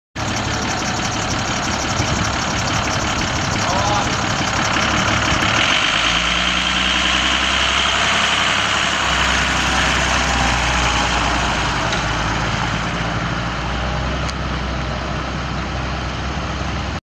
2008 Workhorse W62 delivery truck for sale at auction | bidding closes May 22, 2019
2008 Workhorse W62 delivery truck
201,676 miles on odometer
4.5L six cylinder diesel engine
Automatic transmission
Heat
Cruise
22'L x 7'8"W body
86" side height
Thieman end gate lift
1,600 lbs capacity
245/70R19.5 tires
Windshield cracked
Ohio title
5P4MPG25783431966
It's located in Harrisonville, MO, and we're selling it to the highest bidder regardless of price.